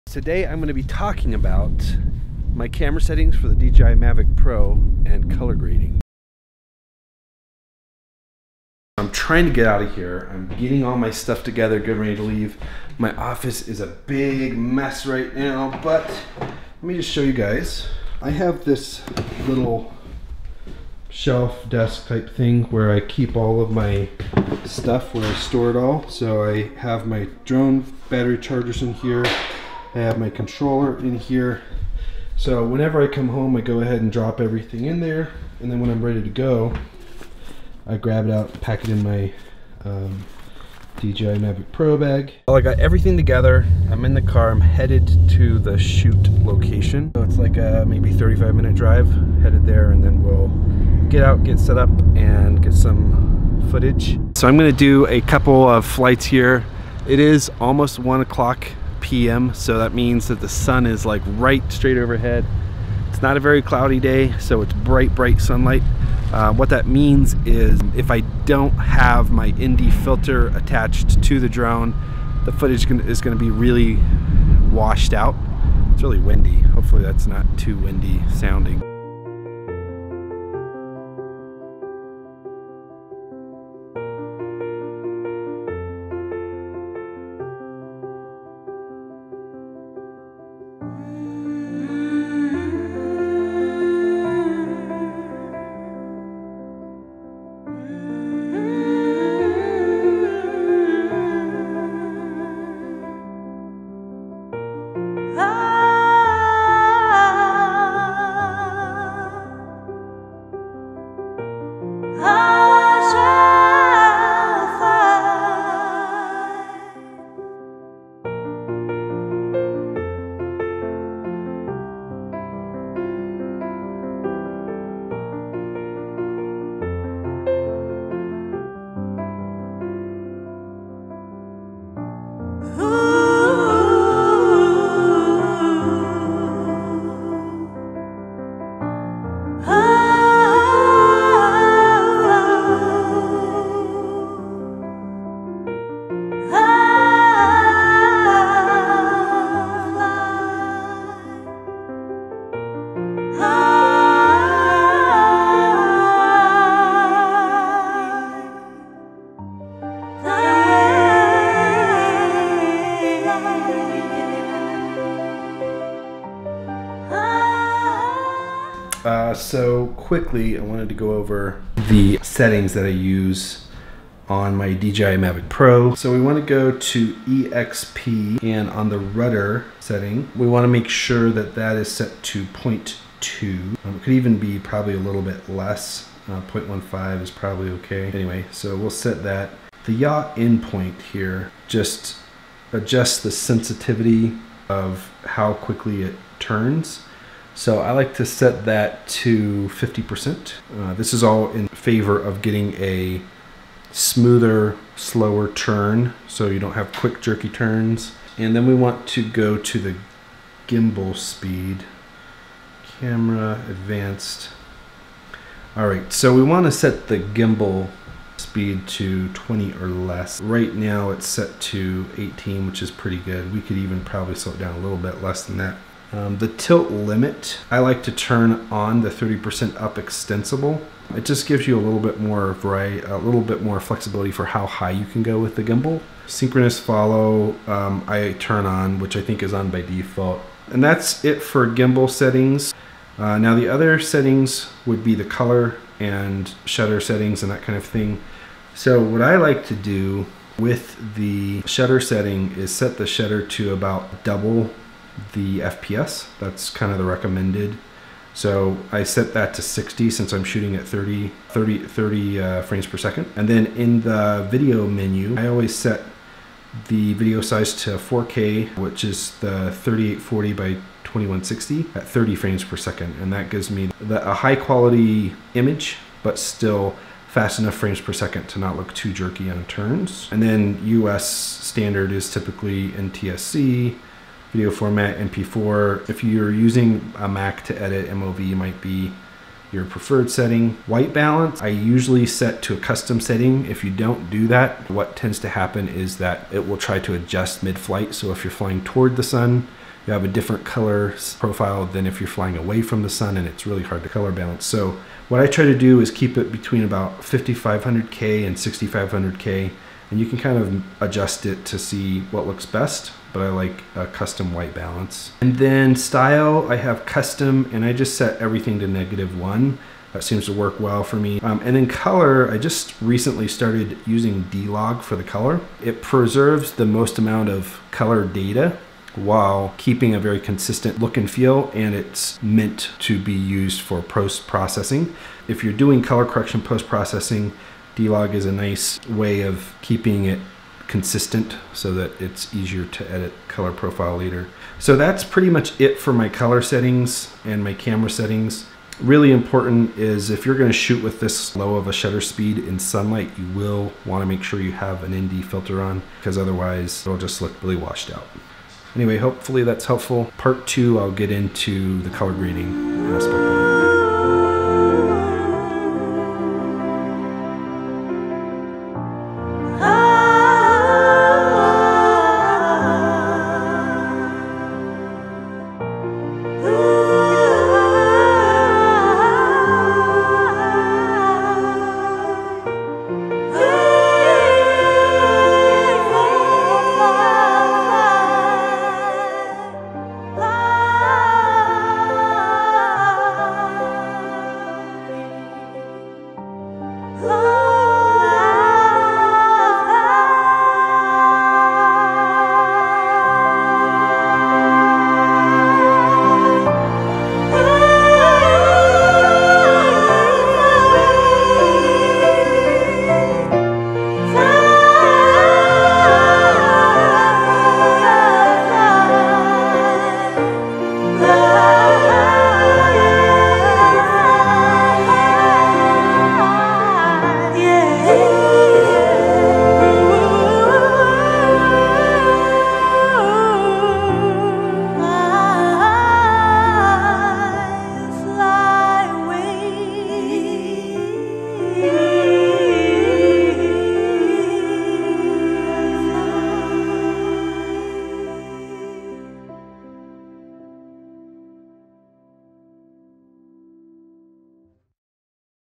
DJI Mavic Pro Best Camera Settings Tutorial - Part 1 of Camera Settings and Color Grading Tutorial
This is not something I'm that great at but so many of you have been asking me about my DJI Mavic Pro camera settings and color grading process that I figured I had to at least take a stab at it. Hope this helps! Part 2 will look at the color grading piece of it.

By the way, I used the DJI Mavic ND16 filter for this. It made a huge difference! Made my color grading way easier. You can find it on the DJI Store